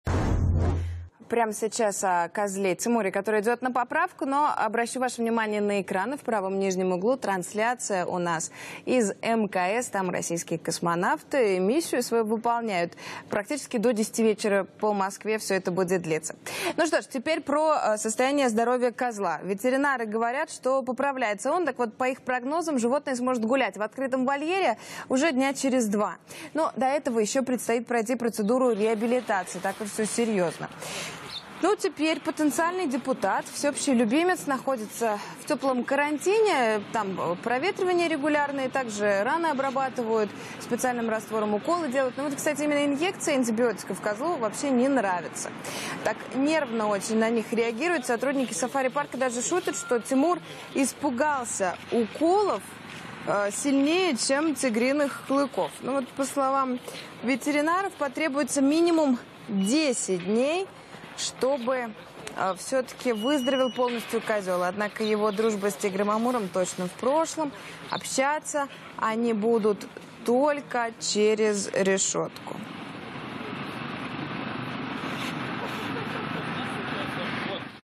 Идущий на поправку козел Тимур боится уколов
Через два дня козел Тимур сможет гулять в открытом вольере